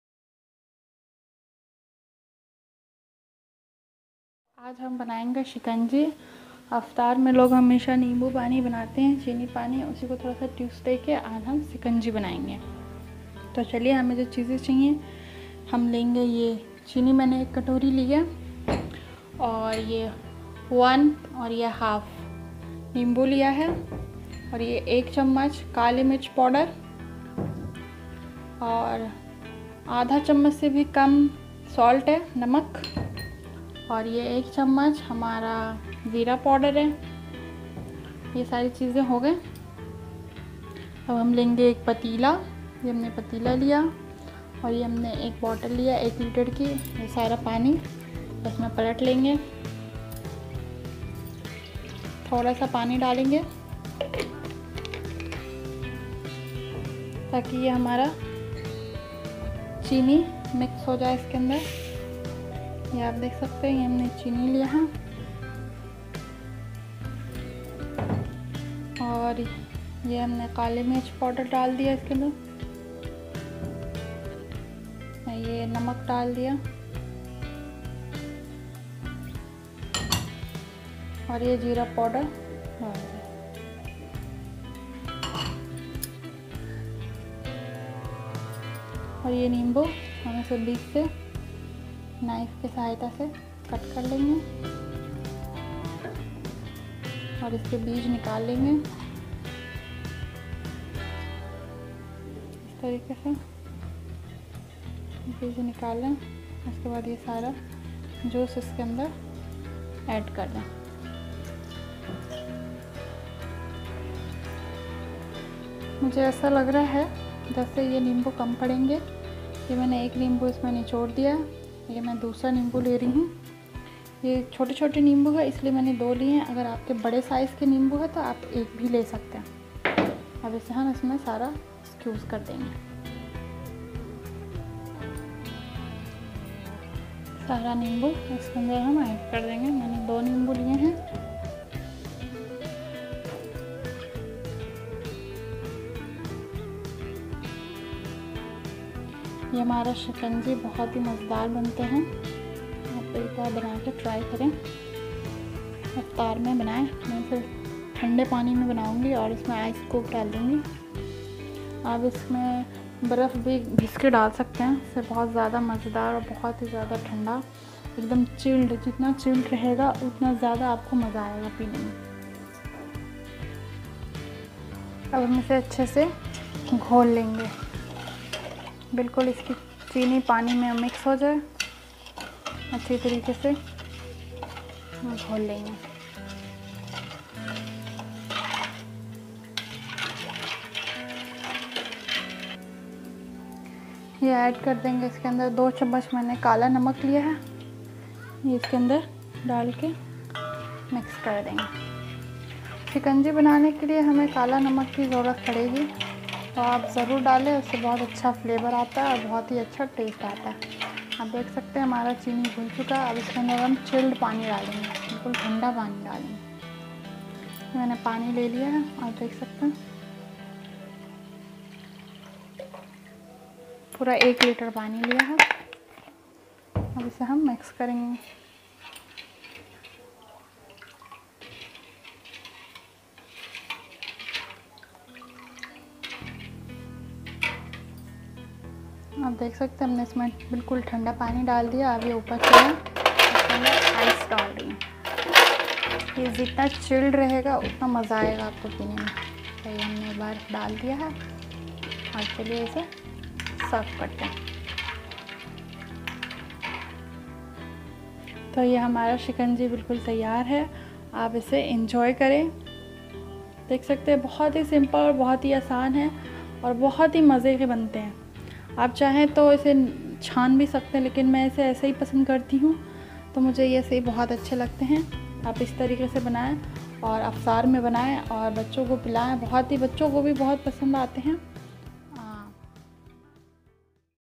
new shikanji sarbat app ise ajj hi try kare ramzan special//shikanji sarbat🍹🍹
how to make shikanji recipe kitchen with noor saba
ADD👇
sugar:1 katori
bada nimbu:1 pes
kali mirch powder:1 tsp
namak:1/2 tsp
jeera powder:1 tsp
kala namak:2 tsp
ice cube
kuch naye tareka sa bante ha
any INFORMATION